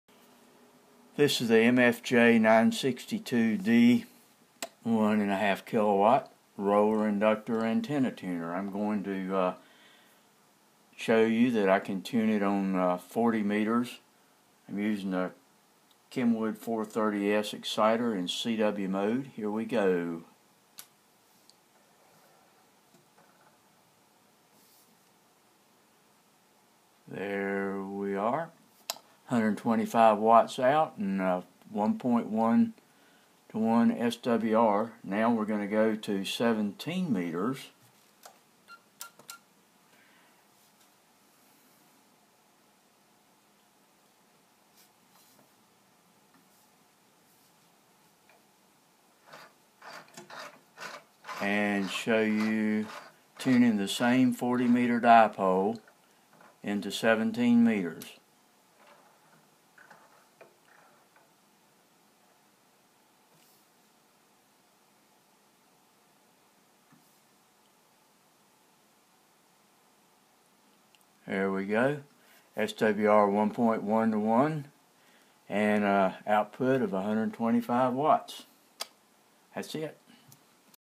MFJ 962 D Antenna Tuner
Manually tuning a 40 m Dipole on 40 m and 17m using a Kenwood TS 430S in cw mode